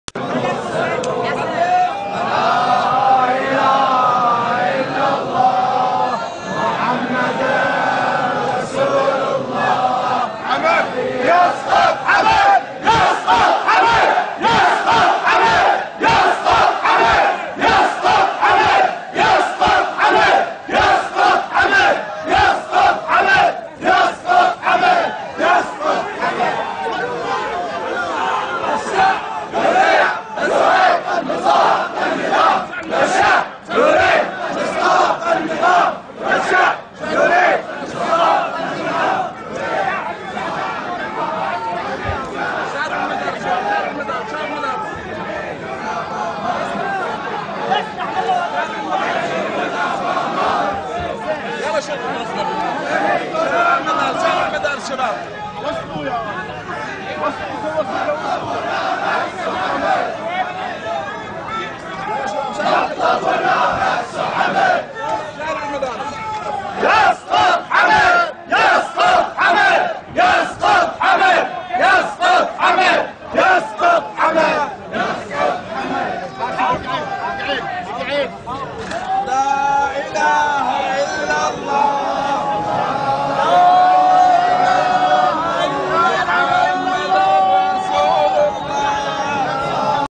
لحظة اخرج جنازة الشهيد جعفر الدرازي من مأتم الديه 28/2/2014 Bahrain
هذا الفيديو أرسل الينا من قبل مواطن لم يذكر أسمه ضمن مشروع المركز الإعلامي "المجتمع الإعلامي" وجاء برسالة يقول فيها: لحظة اخرج جنازة الشهيد جعفر الدرازي من مأتم الديه اليوم الجمعة (28 فبراير/شباط 2014).